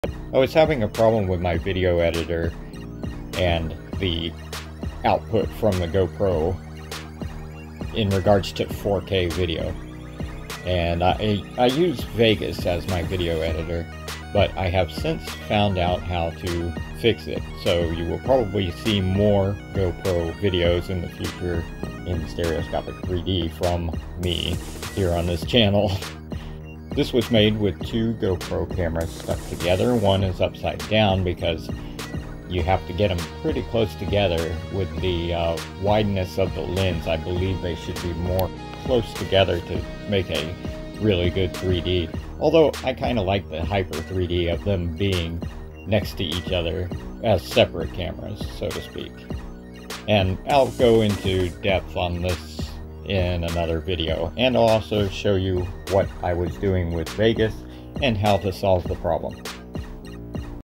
Revisiting GoPro 4K Stereoscopic 3D
I have been frustrated with Vegas Pro when trying to make 3D videos with my GoPro setup, but I discovered a solution that I will share soon!
While you wait you can check out this 4K 3D video!